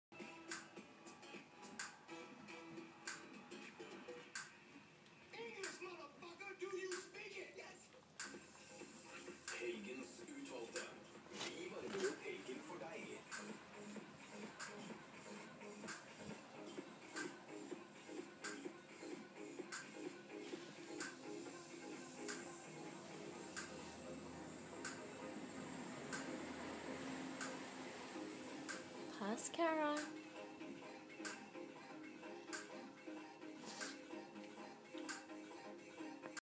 Cara the Ridgeback grooming Blizz the Barockpinto
Cara washing Blizz's dirty back legs and Blizz doesn't care :)